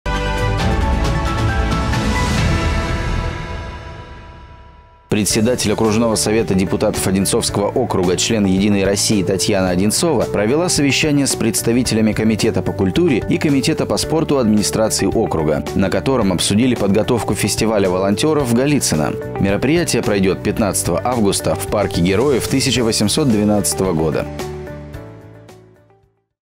Подготовка фестиваля
Председатель окружного Совета депутатов Одинцовского округа, член «Единой России» Татьяна Одинцова провела совещание с представителями комитета по культуре и комитета по спорту администрации округа, на котором обсудили подготовку фестиваля волонтеров в Голицыно. Мероприятие пройдет 15 августа в парке Героев 1812 года.